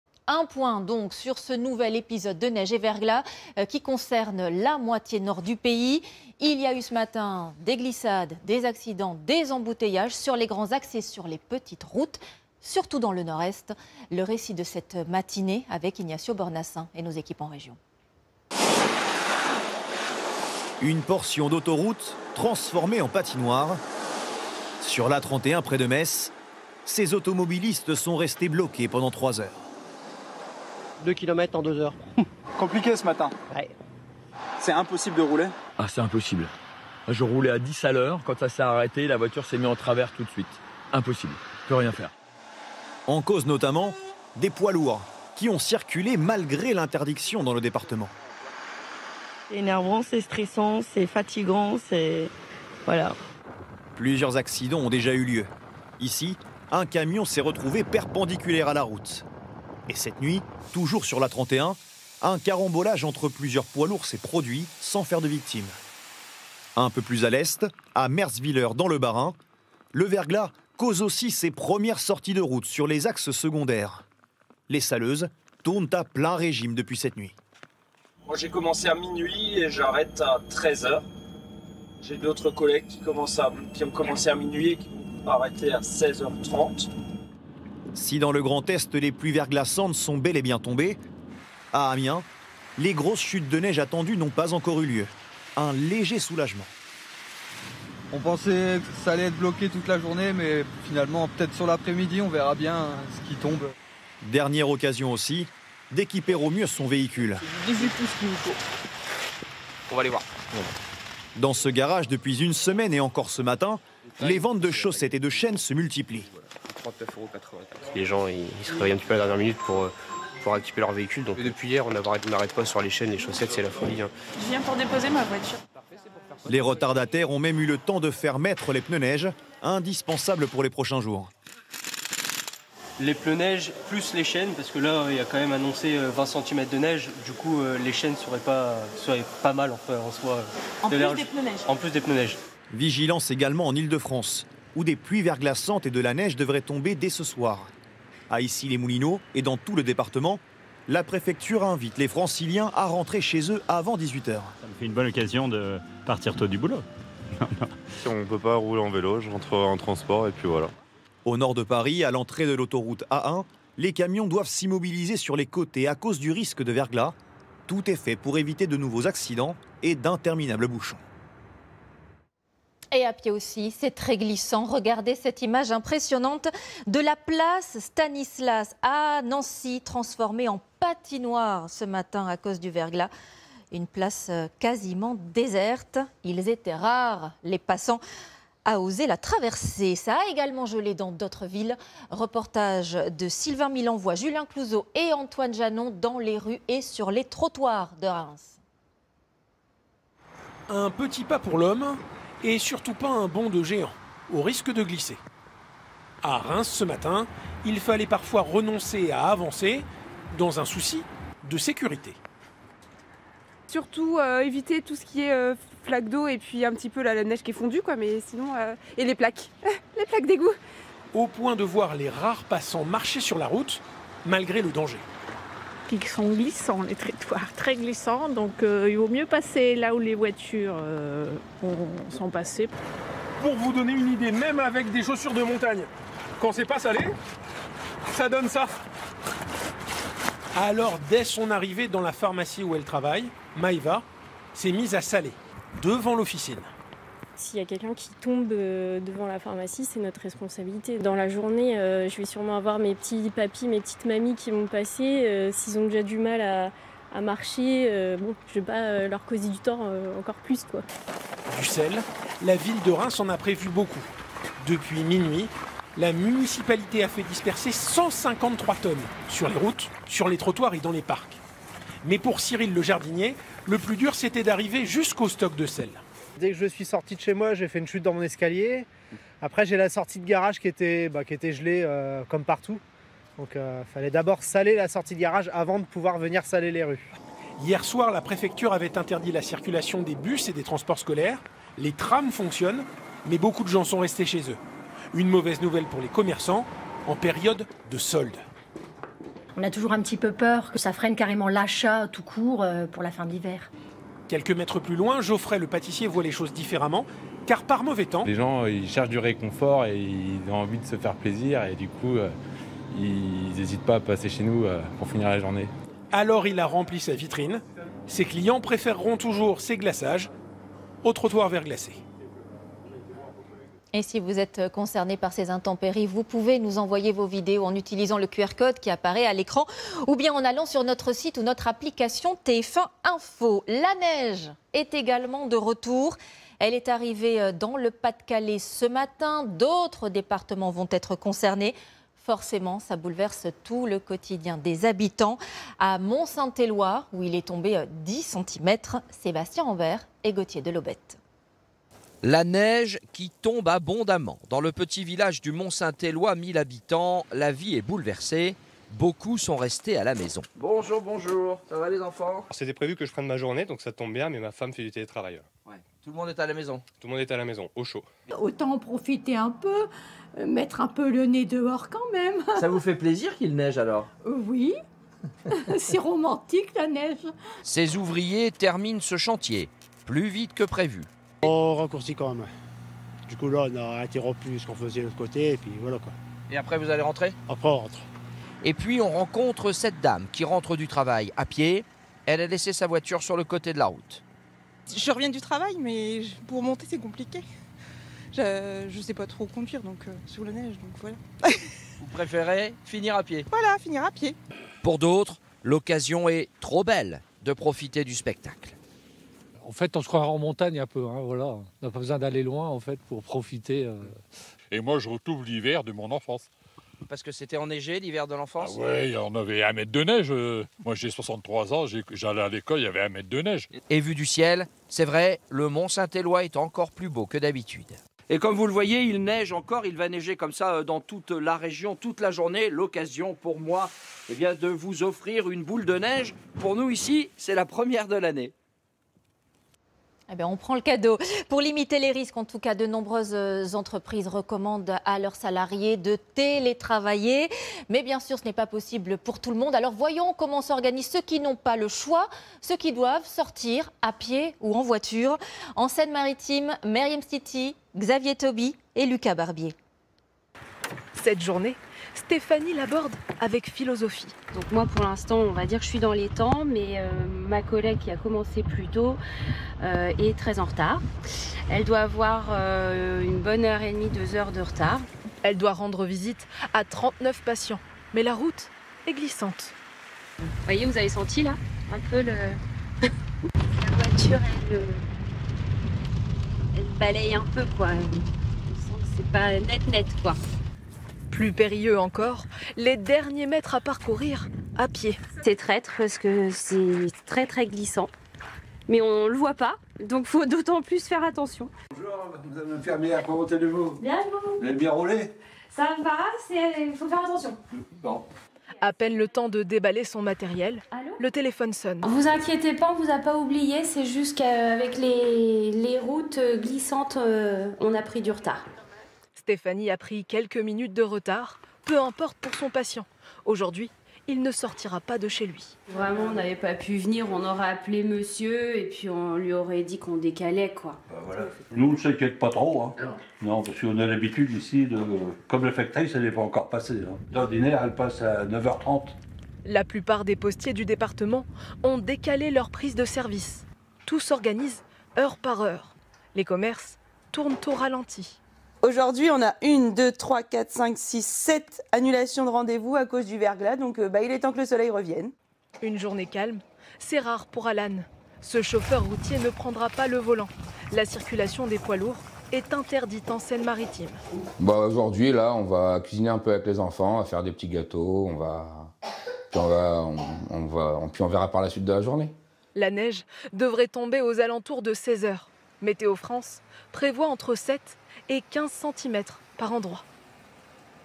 Neige, verglas : glissades, accidents et embouteillages sur les routes
❄️ Trottoirs verglacés, rues impraticables, places transformées en patinoires... Attention aux accidents en voiture ou à pied sur les deux-tiers nord du pays en vigilance orange "neige et verglas" et "pluie-inondation."